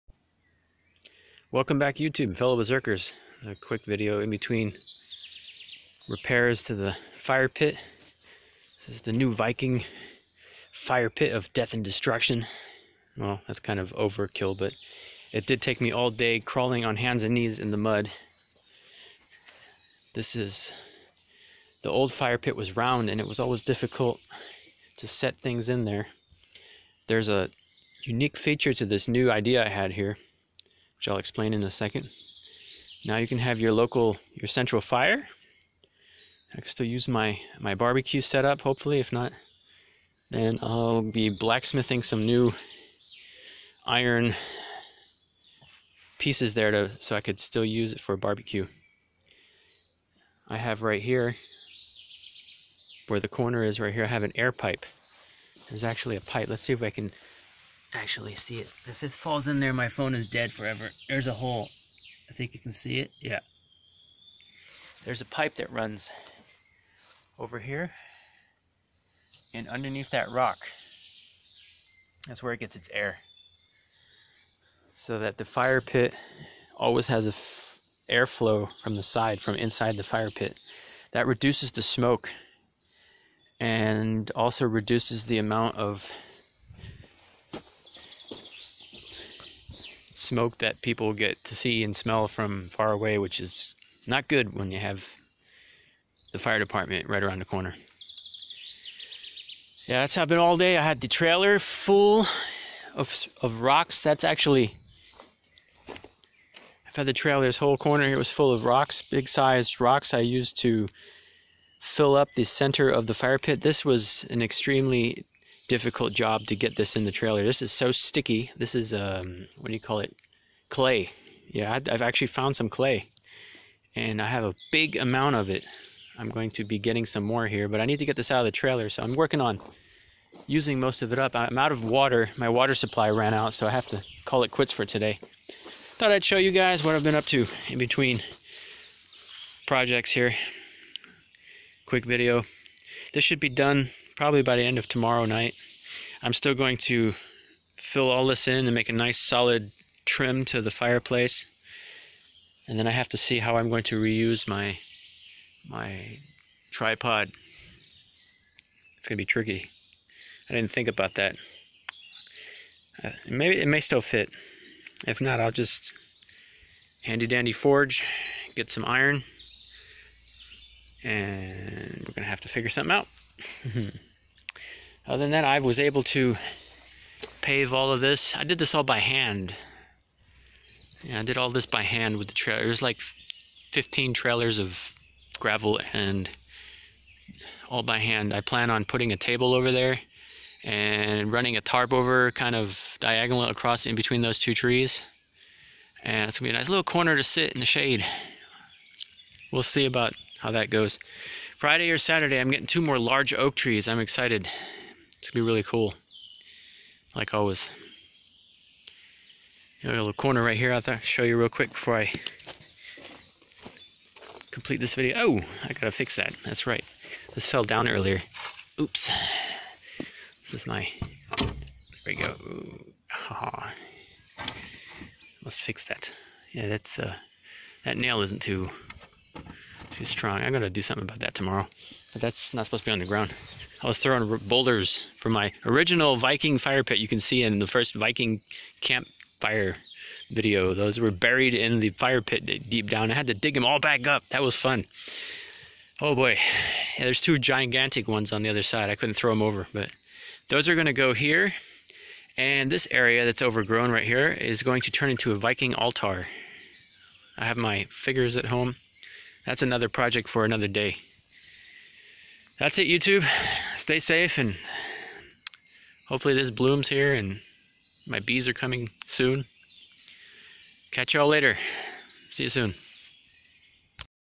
Viking Style Fire Pit
This is the new Fire style  pit of the Viking camp. Less smoke and much more versatile.
I used clay and some rocks that i foraged from the surounding are to make the mold of the fire pit. My goal was to make a fire pit that could reduce smoke and be used for many purposes.

Stay safe, see you soon!